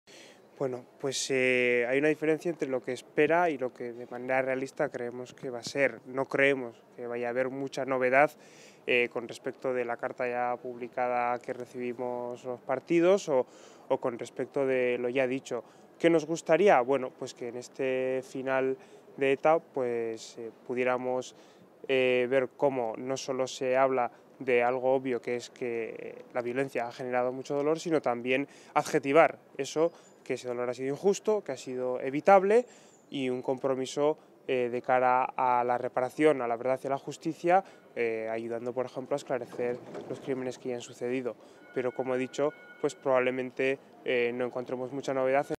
E-Podemos no cree que haya mucha novedad en el comunidado ETA
El portavoz de Elkarrekin Podemos, Lander Martínez, que ha afirmado que aunque le gustaría que el anuncio definitivo sobre la disolución de ETA incluyera un reconocimiento de la "injusticia" del dolor causado por el terrorismo y un "compromiso" con el esclarecimiento de todos los crímenes de la banda, el "realismo" le lleva a pensar que no van a producirse "muchas novedades".